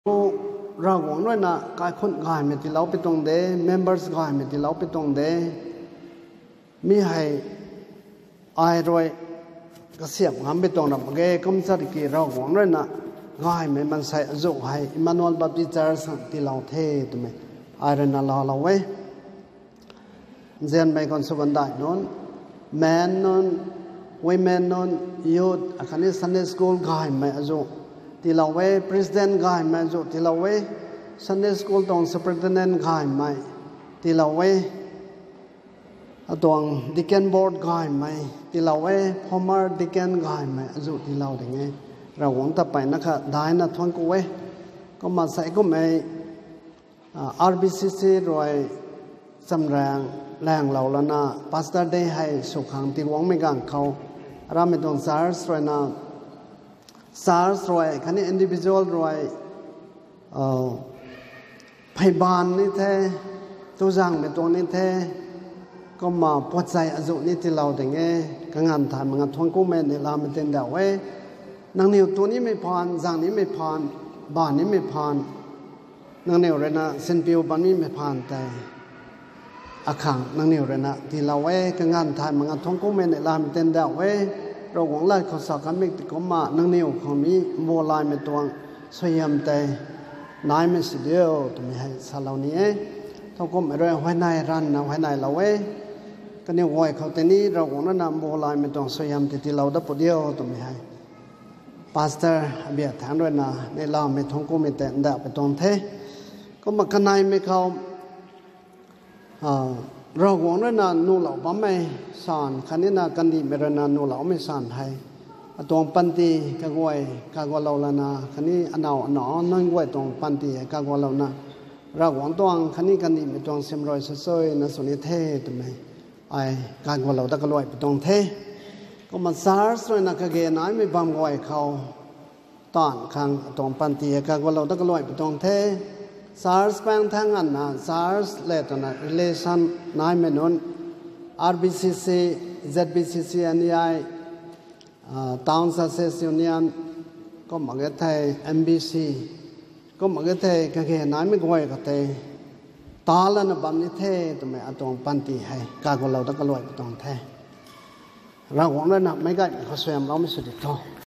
Pastor's Day Emmanuel Baptist Church Tamenglong "Rigaanvpouc" - Guingopou Gonmei Rongmei
Ezekiel - 33:7
"Maansei nacle, ai rui nang ta Isaraelmei kandic tuang rigaanvpouc khang suamclou the. Kumhpui zaeng rui ramh thay kan khang ai rui samei lat tei nang rui kaniw khou salou nina e.